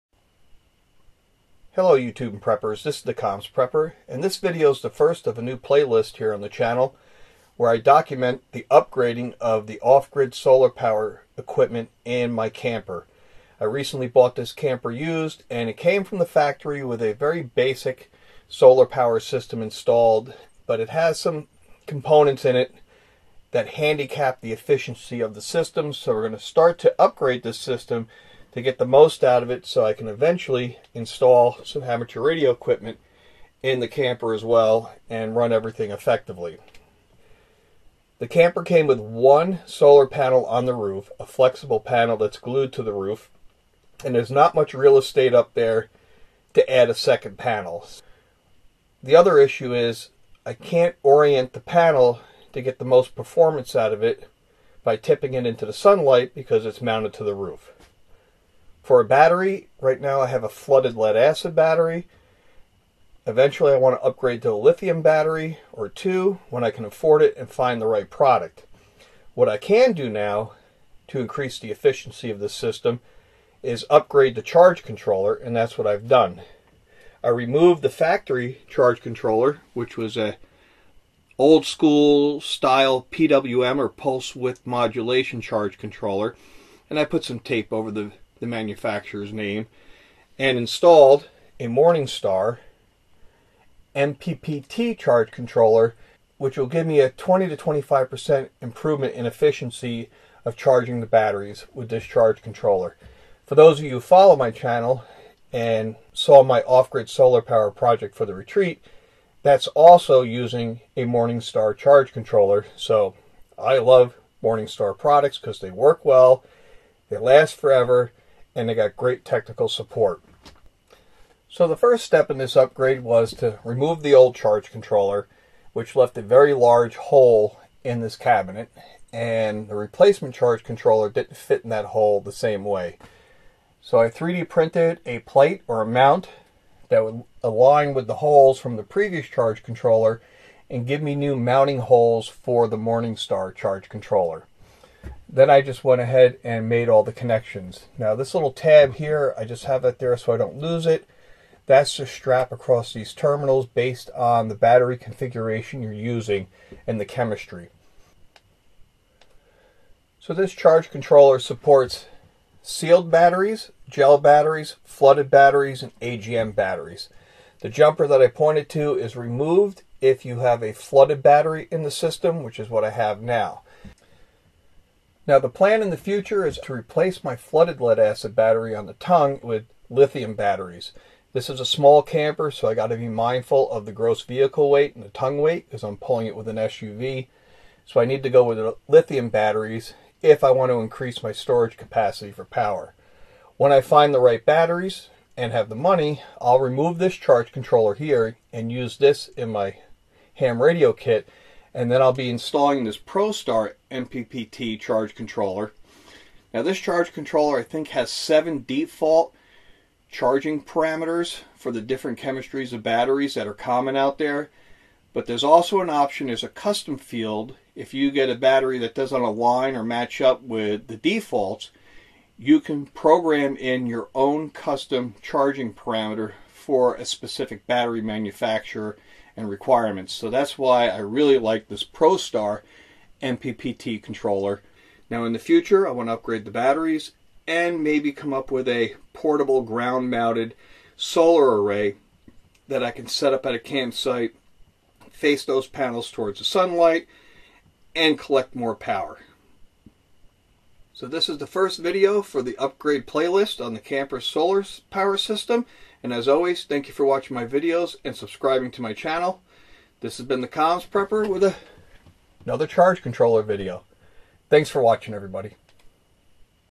R-POD Camper Solar Power Upgrade (Morningstar Charge Controller)
Upgrade of R-POD factory solar power charge controller from PWM to MPPT controller to increase power collection.